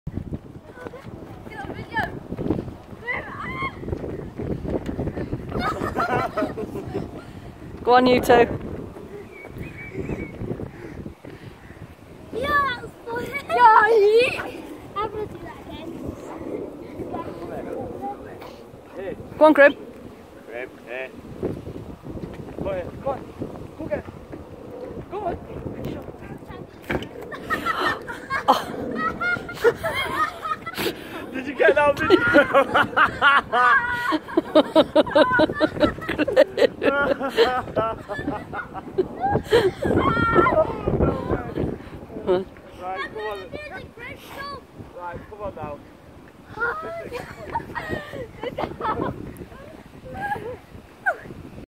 Doggo Faceplant || ViralHog
Occurred on June 6, 2016 / Mere, England

"My son and his friend together with our dog grim. Playing around on a dog walk at mere. The kids were jumping on and off the walkway with grim copying when grim jumped straight into the side of the walkway."